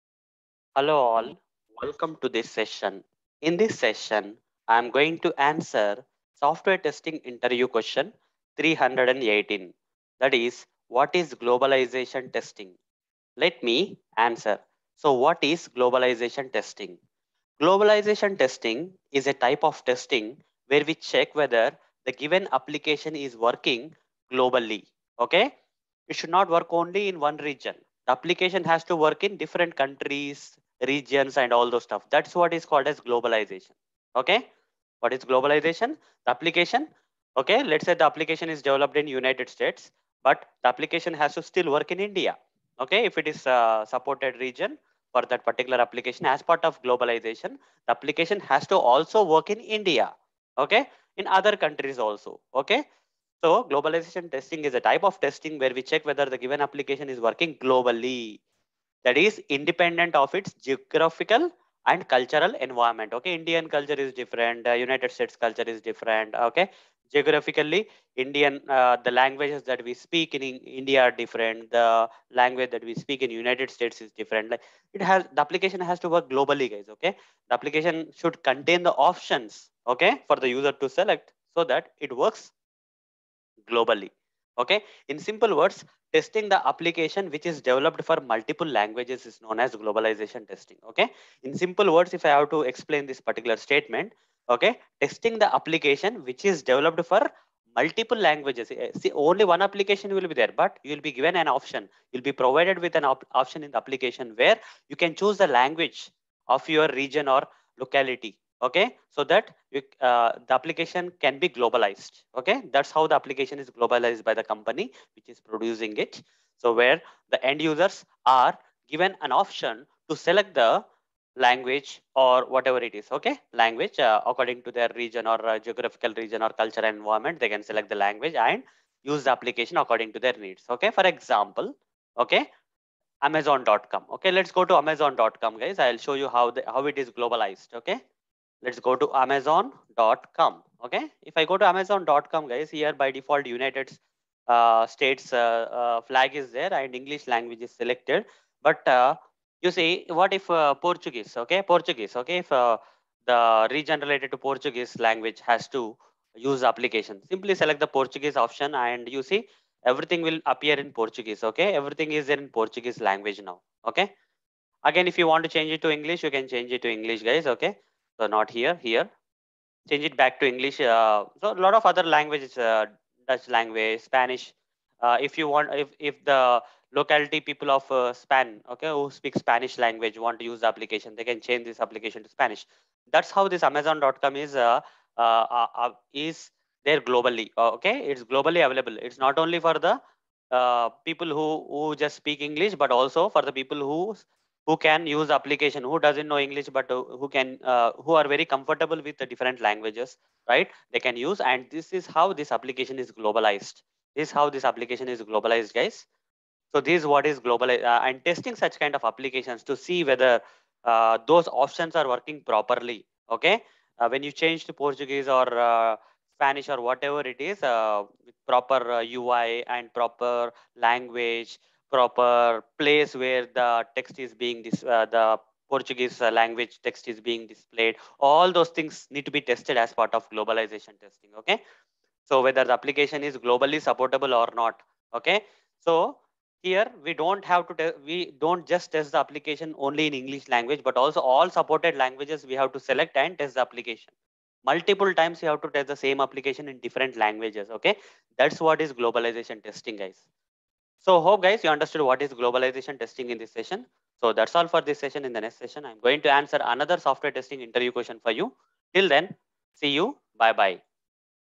What is Globalization Testing? (Software Testing Interview Question #318)
In this session, I have answered What is Globalization Testing?